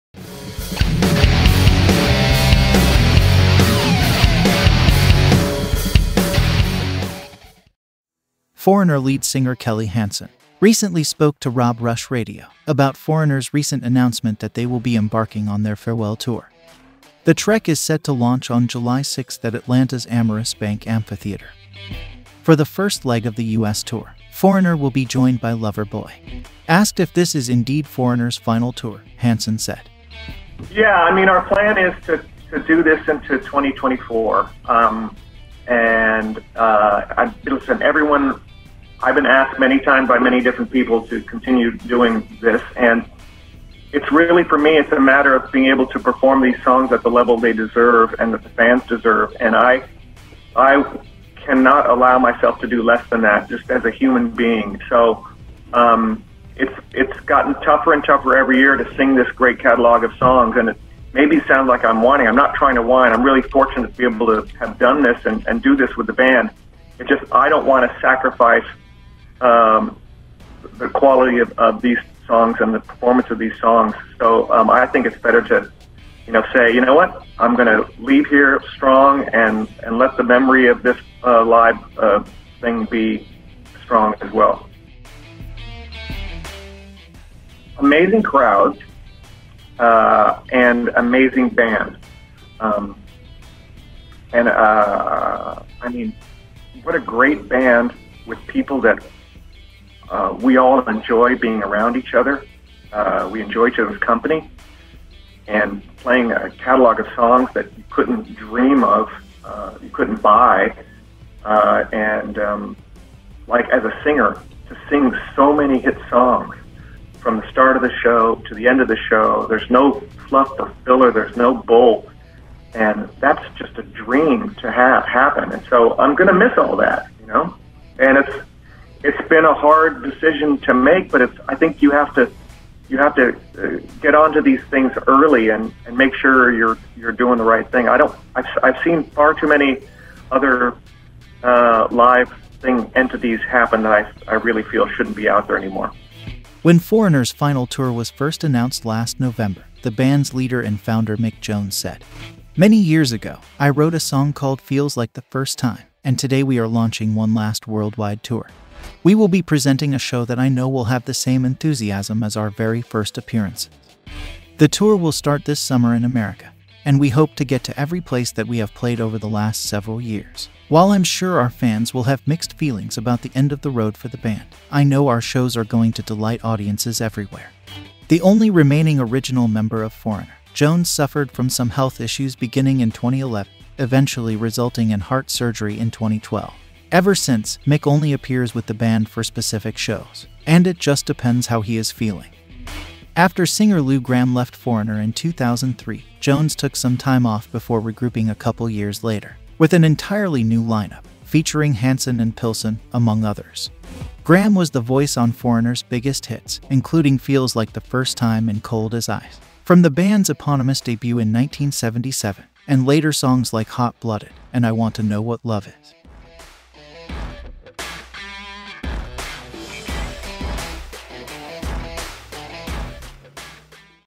⭐"Kelly Hansen's Struggle to Sing Foreigner's Classics: The Truth About the Band's Final Tour"🎤
FOREIGNER lead singer Kelly Hansen spoke to Rob Rush Radio about the legendary rockers' recent announcement that they were embarking on their farewell tour.

‘LIVE LIFE LOUD’.